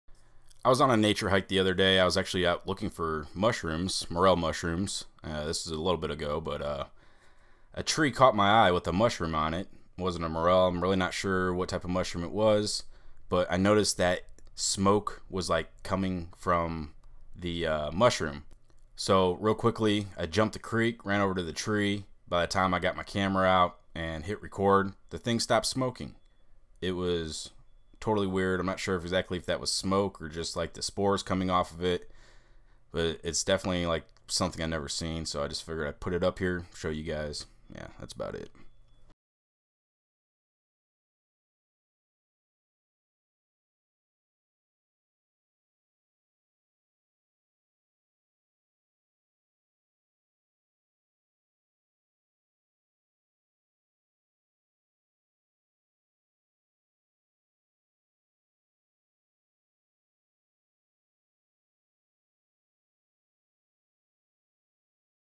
Smoking Mushroom?
I was on a nature hike and I notcied a mushrrom on a tree across the creek and it was smoking. As soon as I jumped the creek and pulled put my camera the mushroom stopped smoking. It was def. putting out something and even more than what I caught on camera. I'm sure there is some scientific reasoning for this but it was pretty neat to see.